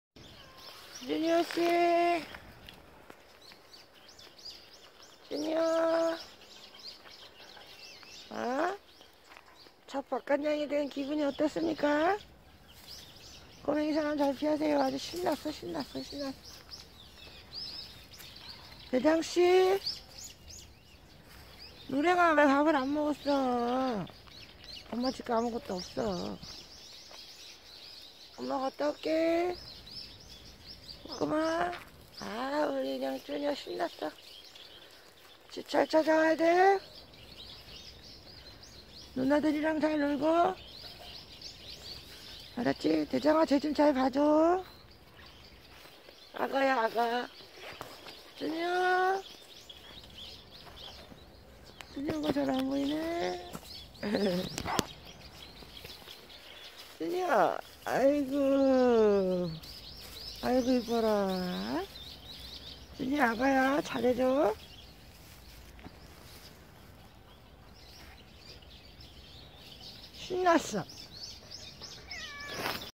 아가냥!,쥬니어가 첫외출을 해서 엄청 신이났어요! The baby cat(Junior) go out first time,he is so exite
캣초딩!엄청잘노는데,워낙사람을 조아해서,손탈까봐,바로데려왔어요!귀엽고,똑똑하고,잘먹어요! ;he is catchoding(he becomes a boy from a child) he is so energetic beyond me and like people very much,so brought him in my place soon,I'm afraid of losing him by someones'hands,he is smart,cute,nimble,good eating,good playing.